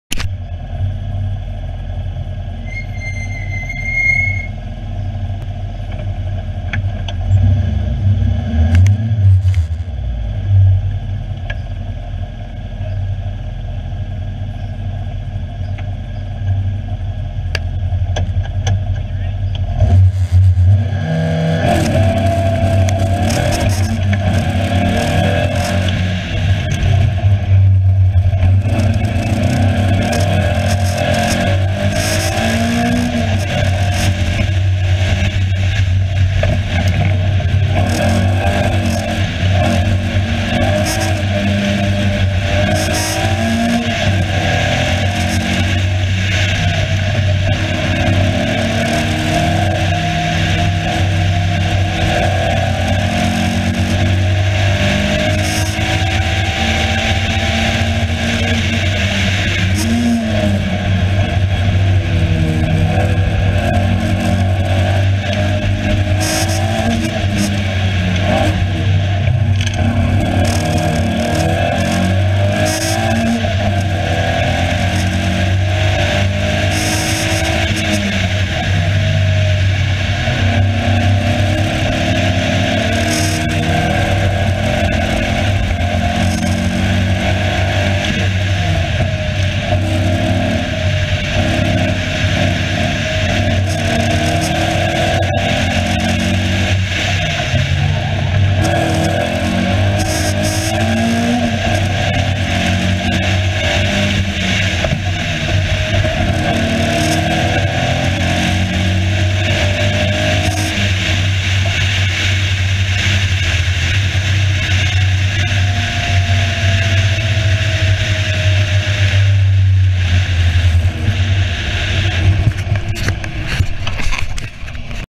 Ruapuna AutoX - First test of 16 channel tyre temp sensors
Some interesting data. Looks like the sensors are fine enough to pick up the grooves in the tyre and the base of the groove doesn't heat up very fast at all.